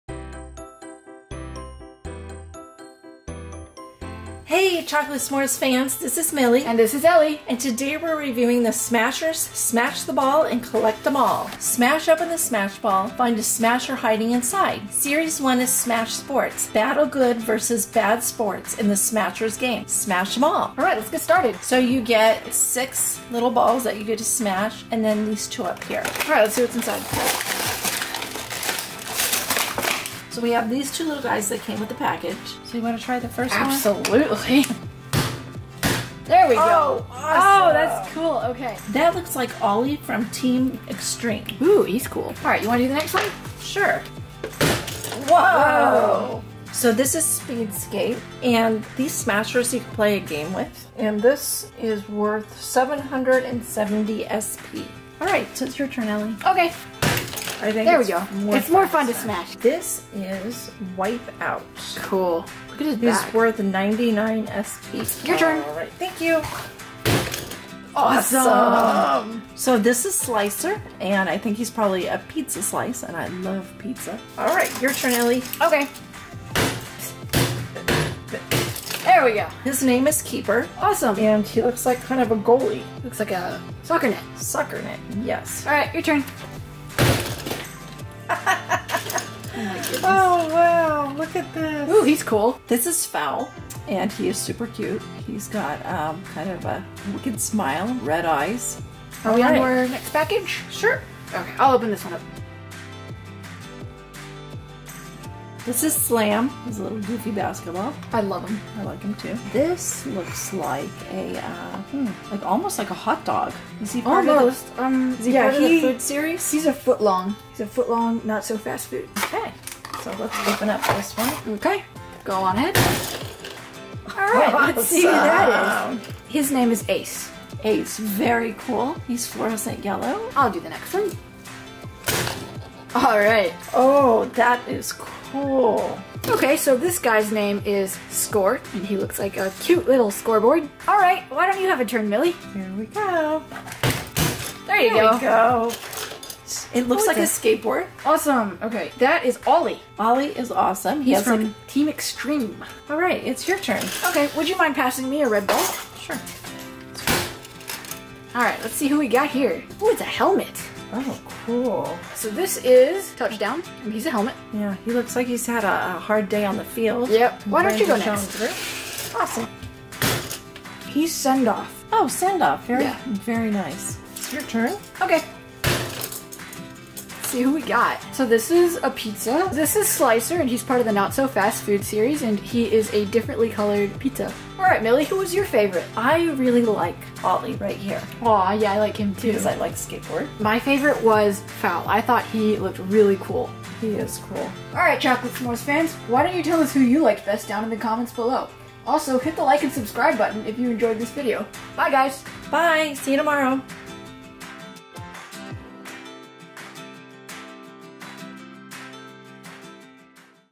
Zuru Smashers
Today we are opening up Zuru Smashers. Let us know down in the comments below which Smasher character you liked the best!

This is Millie and Ellie. We love to open and review toys! Come join the fun at 9:00am every day!